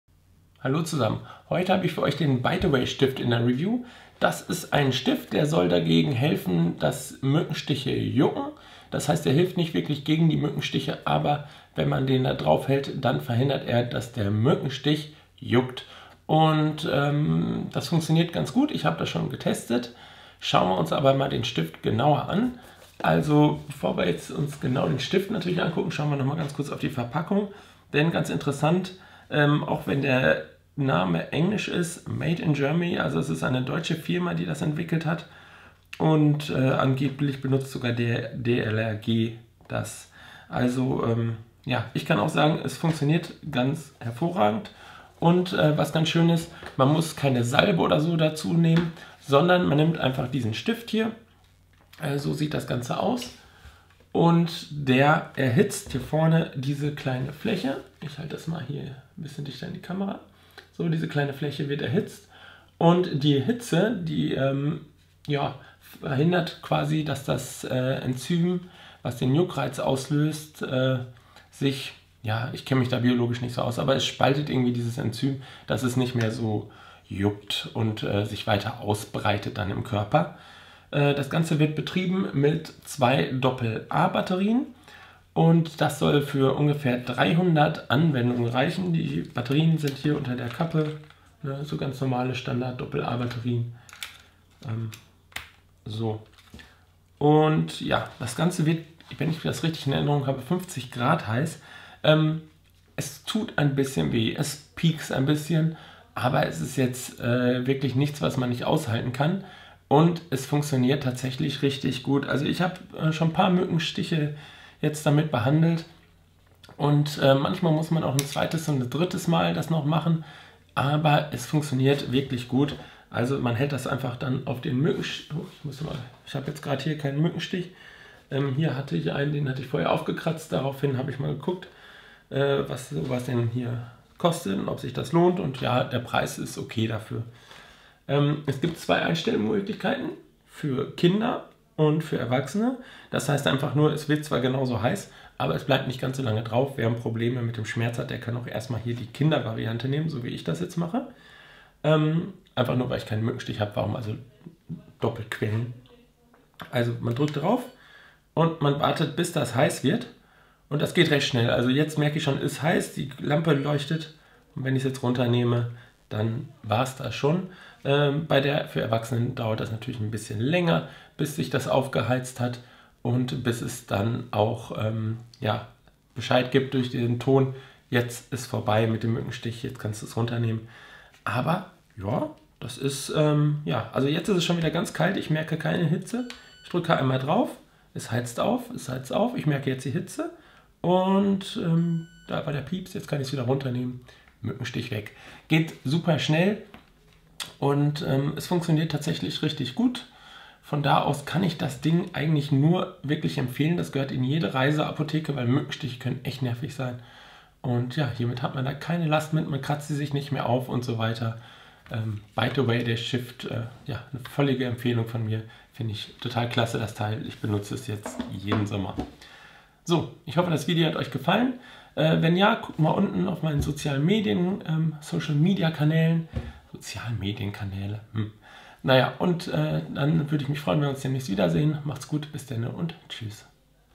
Kein Jucken mehr bei Insektenstichen - Bite Away
Insektenstiche sind lästige Begleiter jeden Sommers.  Wäre es nicht schön wenn sie nicht jucken würden? Genau hier setzt Bite Away mit einer Idee aus der Kategorie "Genialität durch Simplizität" an.